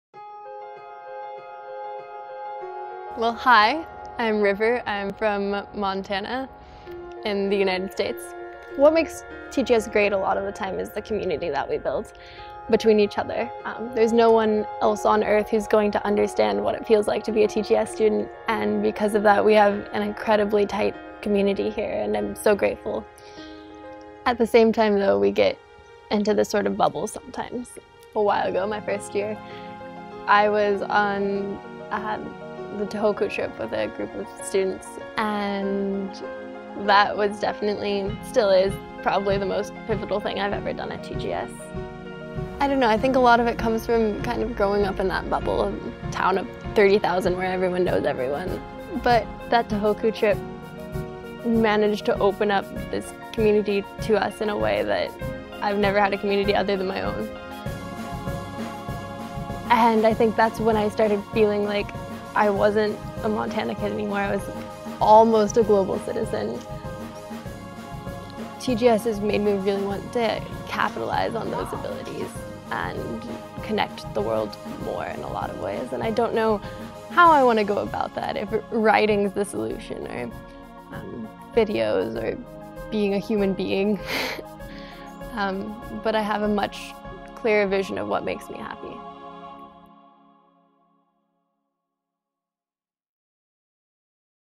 River Wittke THINK Global School Testimonial
Class of 2016 graduate River Wittke reflects on her time at THINK Global School